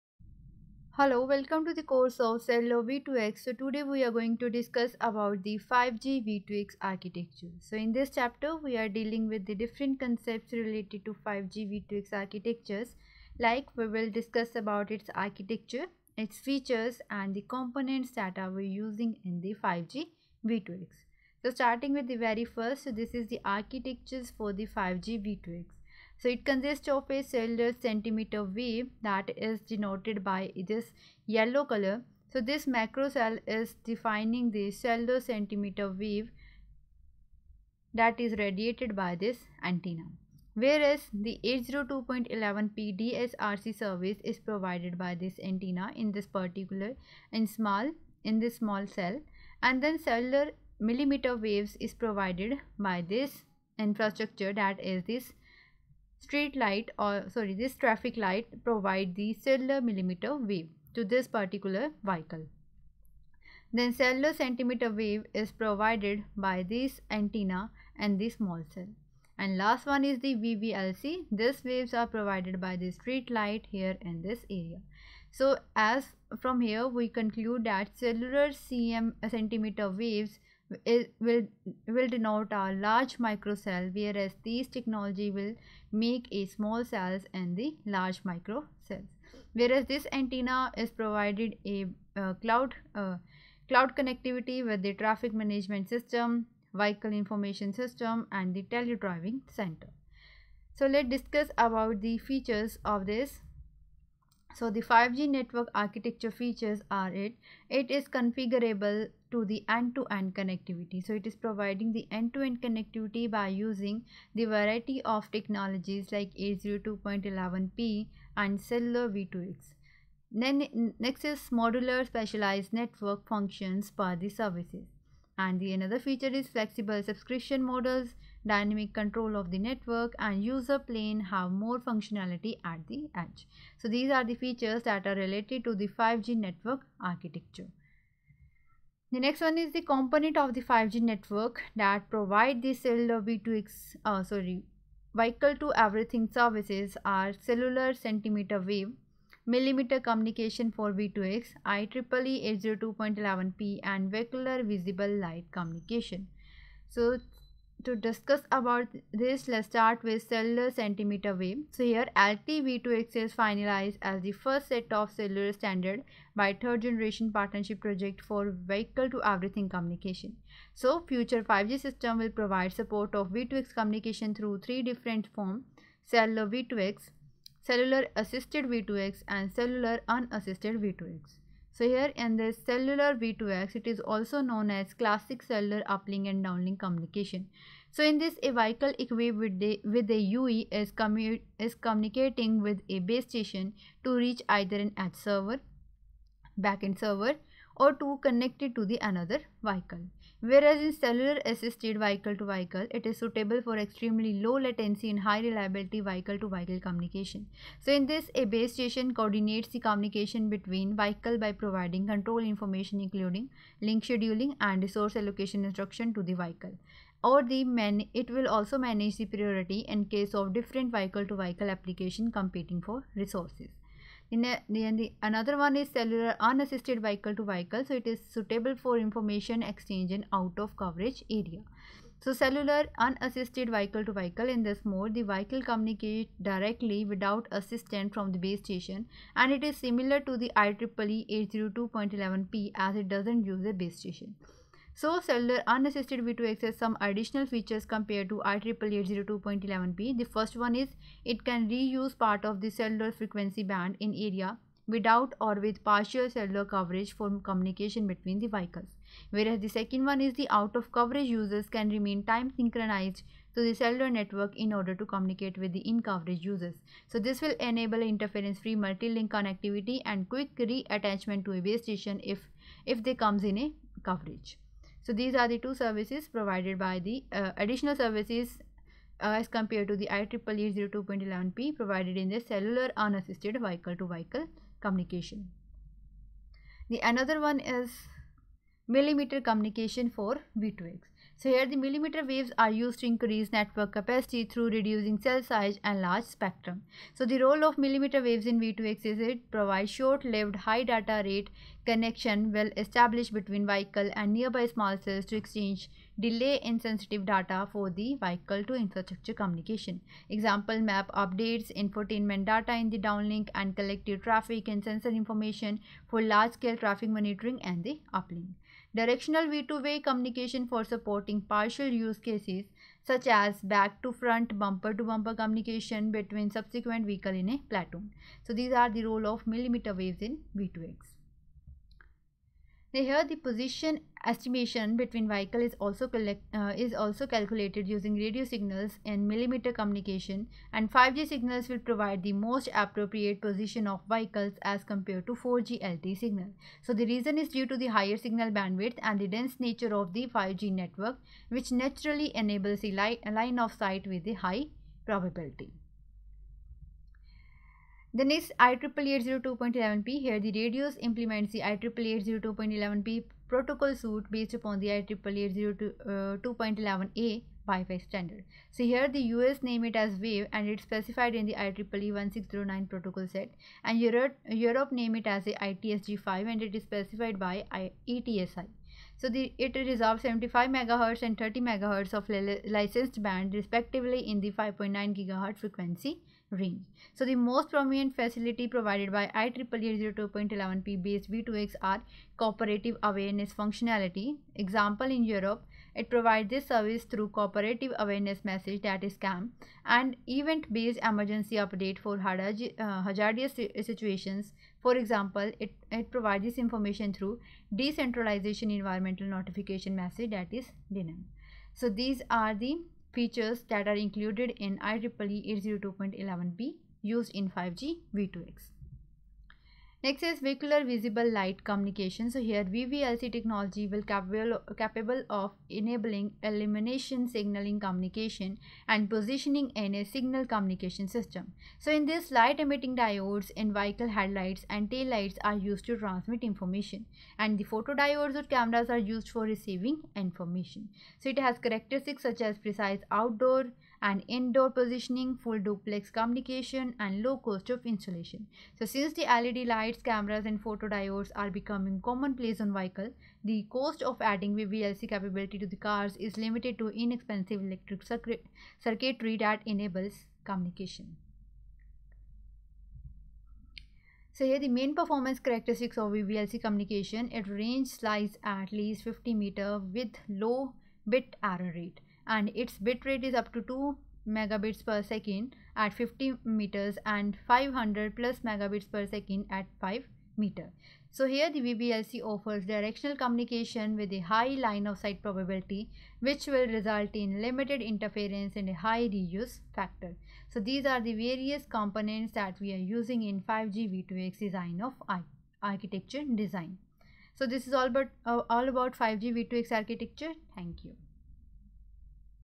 5G V2X Architecture
This video covers Vehicle-to-everything (V2X) communication, 5G V2X Architecture, 5G Network architecture features, cellular centimeter waves, millimeter communication for V2X, IEEE 802.11p, vehicular visible light communication,
Vehicle-to-everything (V2X) communication is essential in providing real-time and highly reliable information flows to enable safe, efficient and environmentally-conscious transportation services and paving the way to connected and automated driving

C-V2X directly connect vehicles to everything—including to each other (V2V), to pedestrians (V2P), and to roadway infrastructure (V2I), and to the network (V2N).